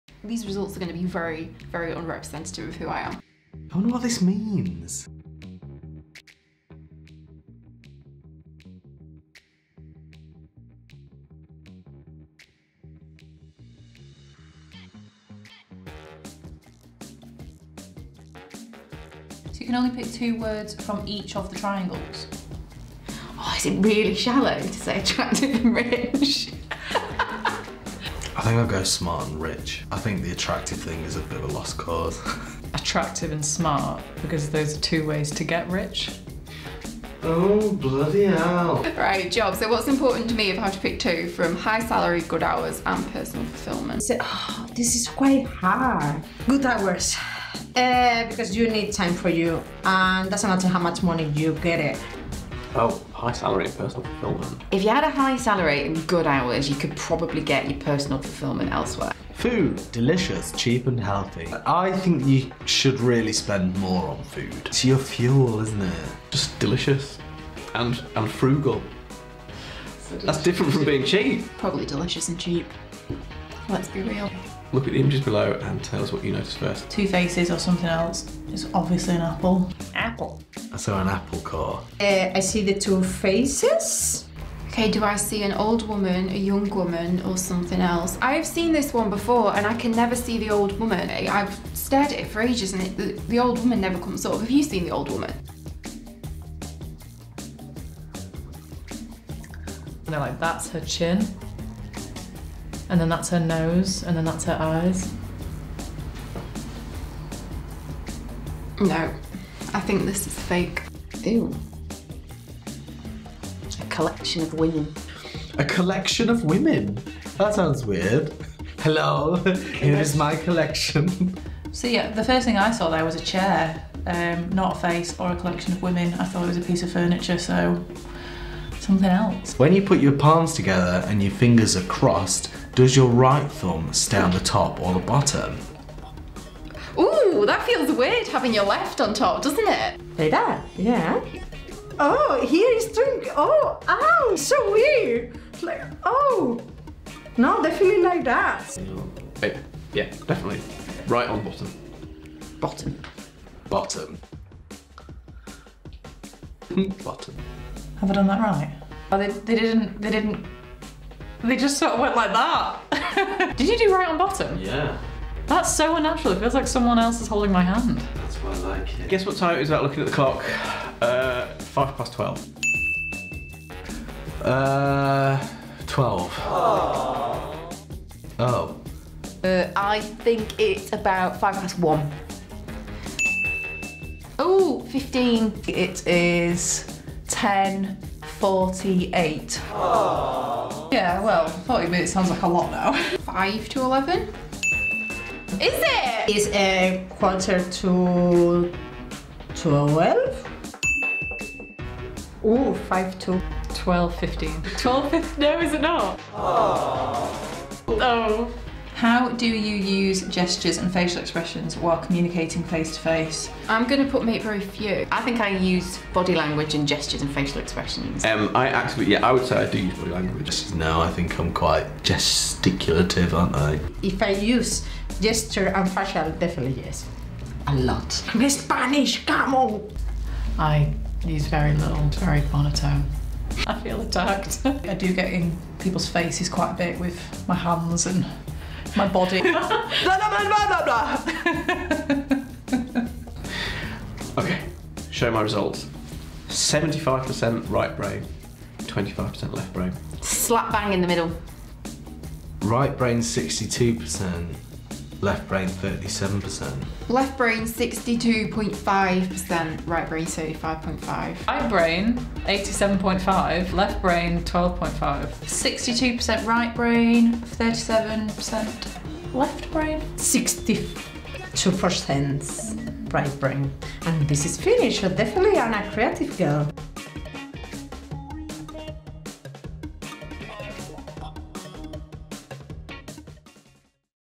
Left Brain vs Right Brain | Workshop Marketing Personality Test
There is a belief that the left side of our brain controls our analytical, logical thoughts, whereas our right side controls creativity and imagination.

So, as a digital marketing agency which certainly requires both types, we decided to test ourselves to see which side was more dominant for each of us!